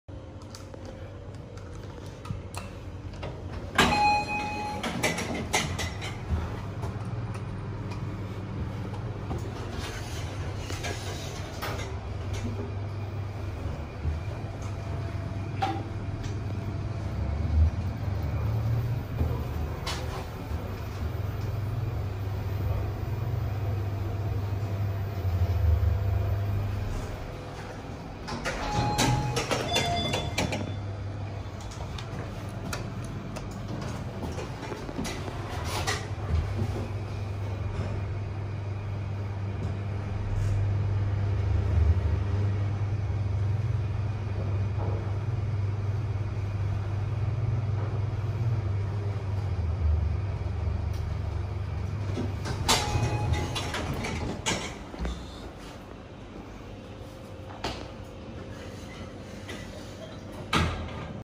OTIS (Ex. Fujitec) MR @ AS6, National University of Singapore.
A Fujitec much like the S/MD blocks that got modernised with the wrong interior door size by Otis.

While AS1 and AS2 did not get lifts, AS3 to AS7 got a mix of brands. Afterwards, some were modernised into Otis while others got replaced totally by Hitachi.